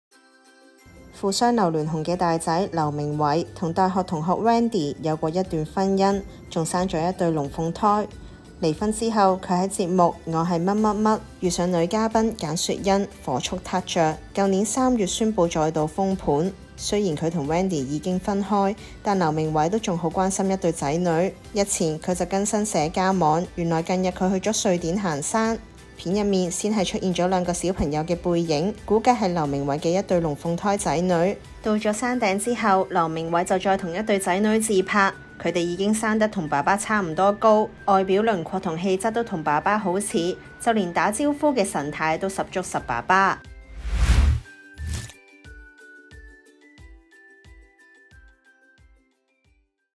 劉鳴煒龍鳳胎罕曝光外表神態似足爸爸 人仔細細同老竇差唔多高｜01娛樂｜劉鳴煒
富商劉鑾雄（大劉）長子劉鳴煒感情路豐富，他與大學同學Wendy有過一段婚姻，並誕下一對龍鳳胎，離婚後戀上金融之花奚倩雯，再戀上氣質主播Keina。劉鳴煒沒有因離婚對婚姻失去信心，在節目《我係乜乜乜》遇上女嘉賓簡雪忻（Joyce）火速撻著，去年3月宣布再度封盤，今年結婚一周年兩人更到智利行足7日山慶祝，相當恩愛。